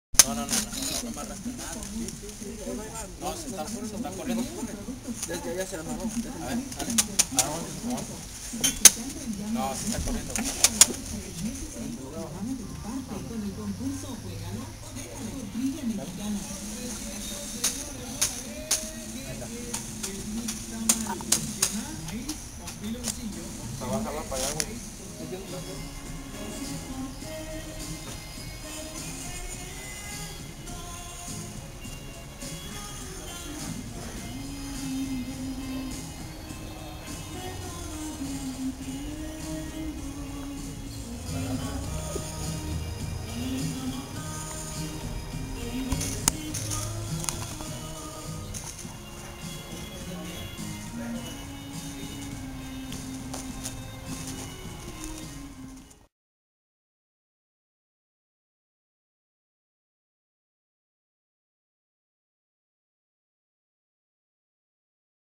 Limestone Steps, Decks, Walls & Fountains
Lexington Job 10-12-07 Limestone Steps, Decks, Walls & Fountains. Site Visit Video  in Bel Air Ca by mason Mitch Stein production mgr./ job foreman. * at the time